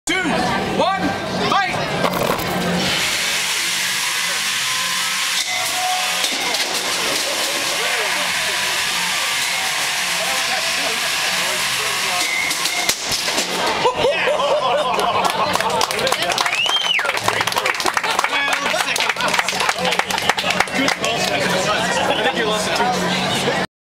Angry Dragan vs Utopia; Kilobts XIV Beetleweights
Utopia's first of two fights against Angry Dragan. Utopia is the drum spinner, Angry Dragan is the full body spinner. As you can see, luck plays a big role in combat robotics when two hard hitting spinners are involved. Angry Dragan took an unlucky bounce directly into Utopia's drum.